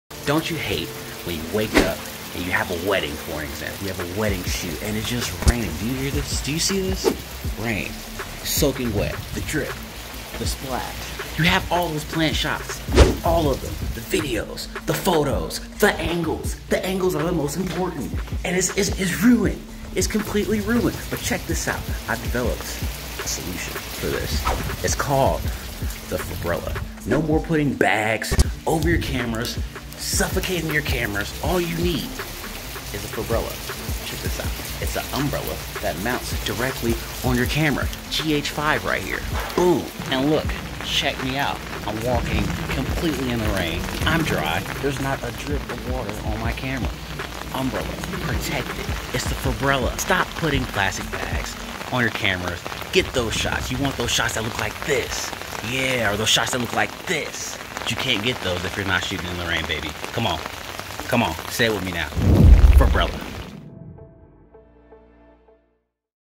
THE 4Brella Camera Rain Cover
This is legit the best way to protect your Dslr camera from the rain.